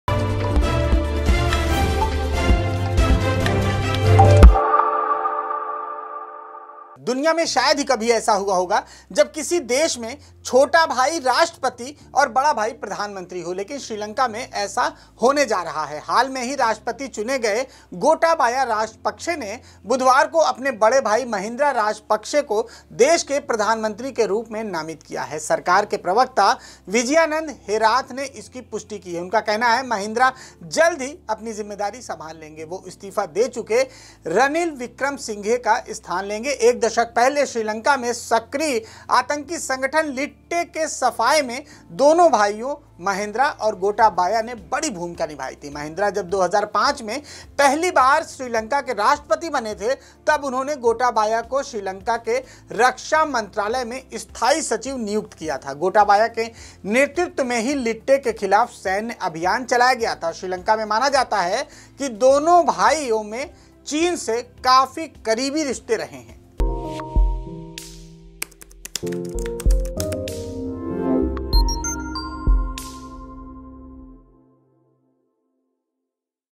दुनिया में पहली बार छोटा भाई राष्ट्रपति तो बड़ा भाई प्रधानमंत्री
Sri Lanka's new President Gotabaya Rajapaksa has named his elder brother Mahinda as prime minister.
Mahinda Rajapaksa - who was president himself between 2005 and 2015, will replace Ranil Wickremasinghe when sworn in on Thursday.
The former prime minister stepped down after his party's presidential candidate lost the election earlier this week..दुनिया में शायद ही ऐसा कभी हुआ होगा, जब किसी देश में छोटा भाई राष्ट्रपति और बड़ा भाई प्रधानमंत्री हो, लेकिन श्रीलंका में ऐसा ही होने जा रहा है। हाल ही में राष्ट्रपति चुने गए गोटाबाया राजपक्षे ने बुधवार को अपने बड़े भाई महिंदा राजपक्षे को देश के प्रधानमंत्री के रूप में नामित किया है।सरकार के प्रवक्ता विजयानंदा हेराथ ने इसकी पुष्टि की है। उनका कहना है कि महिंदा जल्द जिम्मेदारी संभाल लेंगे। वे इस्तीफा दे चुके रनिल विक्रमसिंघे का स्थान लेंगे एक दशक पहले श्रीलंका में सक्रिय आतंकी संगठन लिट्टे के सफाए में दोनों भाइयों महिंदा और गोटाबाया ने बड़ी भूमिका निभाई थी। महिंदा जब 2005 में पहली बार श्रीलंका के राष्ट्रपति बने थे, तब उन्होंने गोटाबाया को श्रीलंका के रक्षा मंत्रालय में स्थायी सचिव नियुक्त किया था। गोटाबाया के नेतृत्व में ही लिट्टे के खिलाफ सैन्य अभियान चलाया गया था।श्रीलंका में माना जाता है कि दोनों भाइयों के चीन से करीबी रिश्ते हैं।